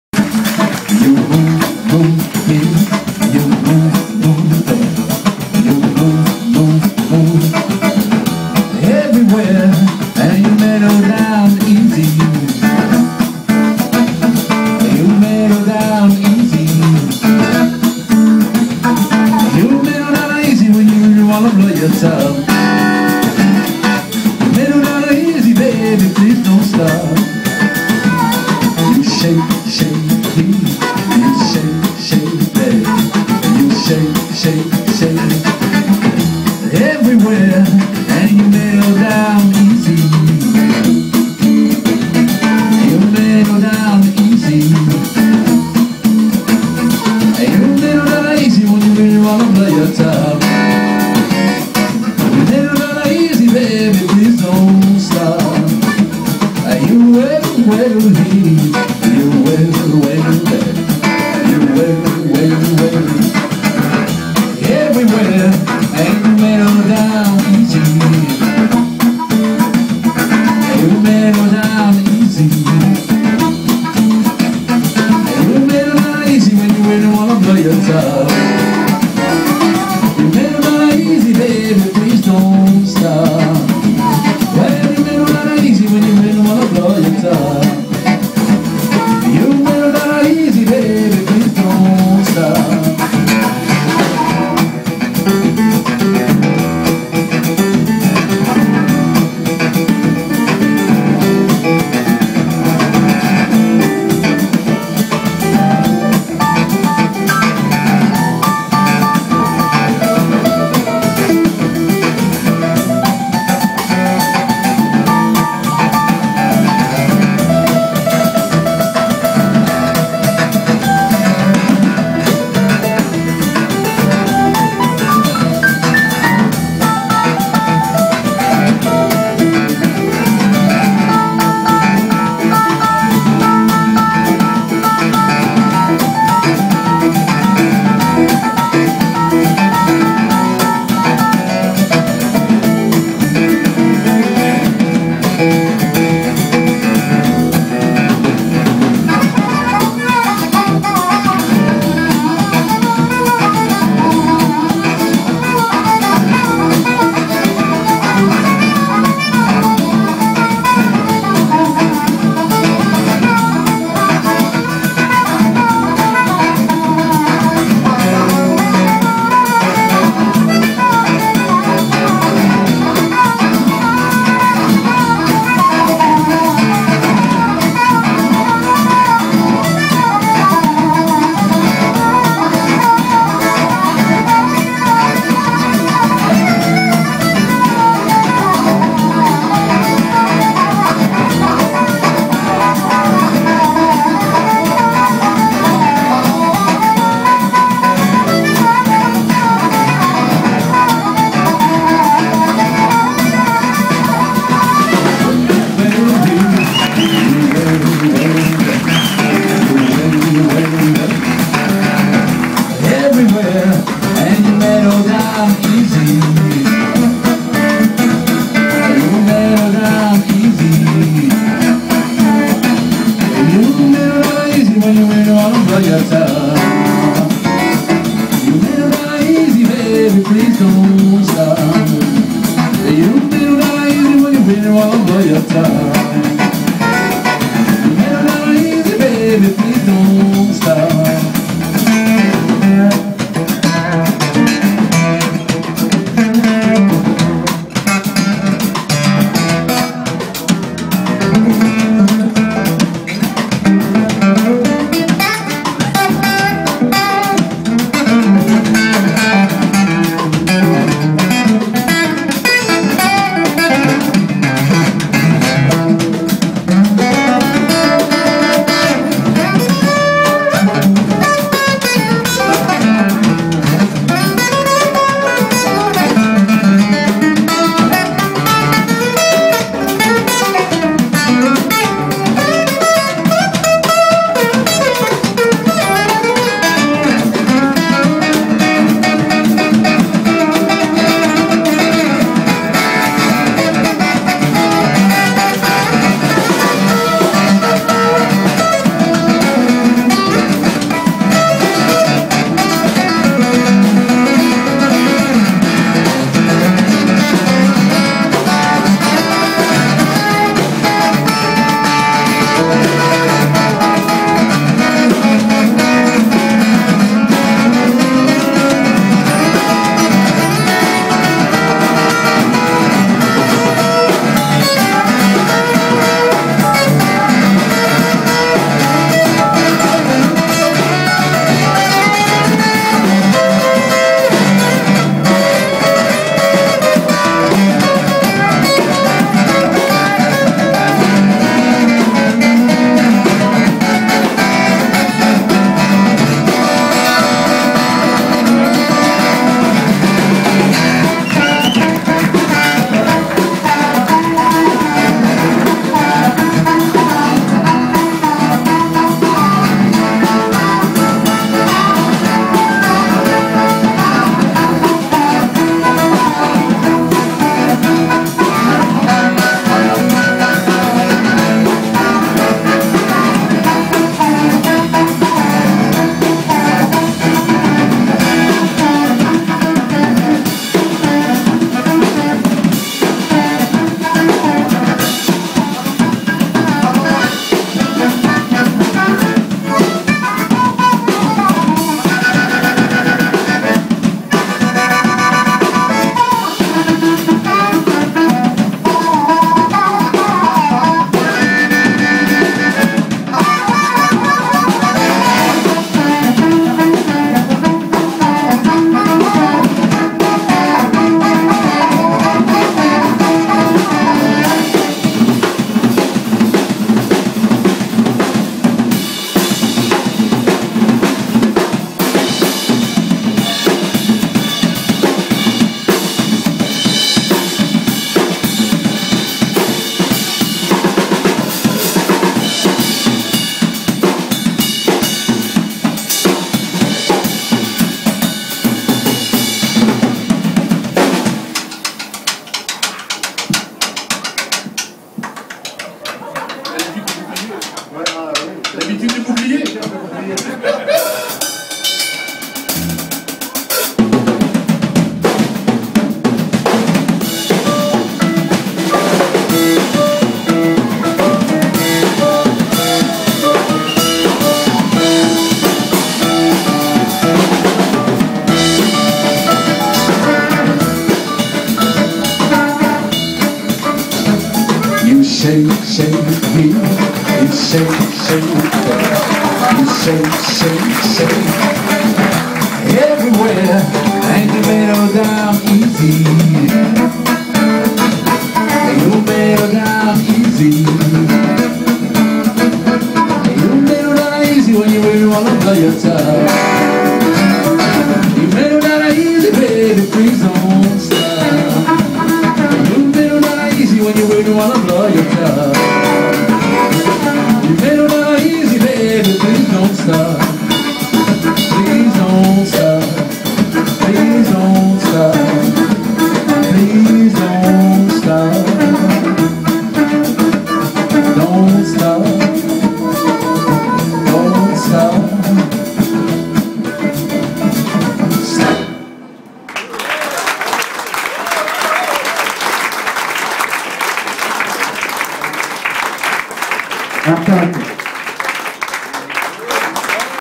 Jean Jacques Milteau and Artak Nersisyan (Mr.Harmonica)
Legendary Jean Jacques Milteau and Artak Nersisyan (Mr.Harmonica)
Utopia Café Concert Paris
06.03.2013